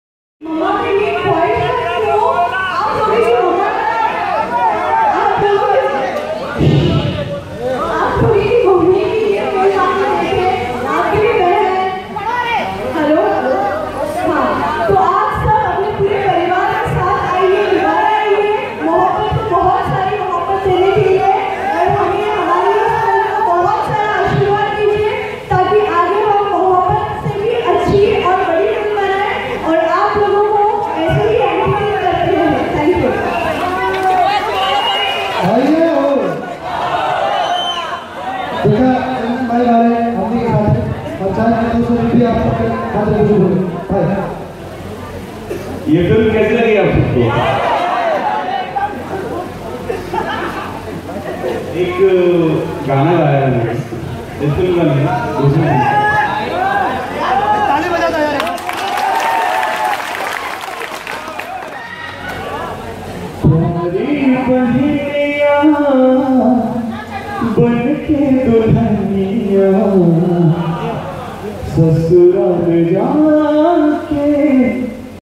Film Mohabbat Team Promoting their Film Mohabbat
Film Mohabbat ka promotion karte najar aayi Maya Yadav, Kajal Yadav, Pradeep Pandey, Chandan Jha aur anye